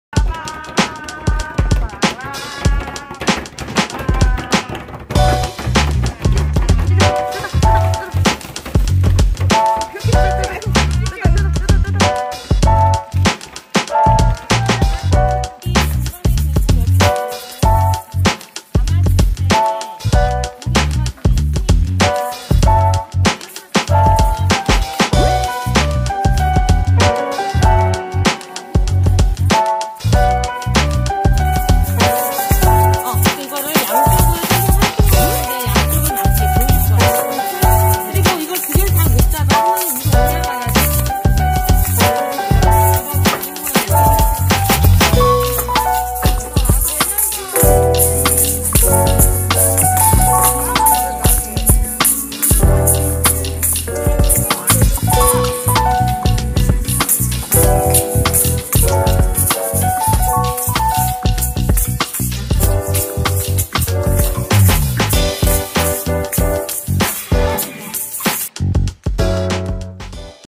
[소울아살라토] 소울친구들과 두근두근 아살라토 언박싱🙊
아살라토  왔또요~~😁😁
작년 봄에 담아둔
장바구니에서 드뎌 탈출~~🙀🙊💥

언박싱은 웨케 즐거움?
소울 친구들도
이젠 "또 새로운거 안와요???"
물어본답니다.

아직 장바구니에 한가득
신박한 애들 많아요ㅋㅋ

오늘 처음 만져봤는데
벌써 댄댄까지
찐 실력자들^^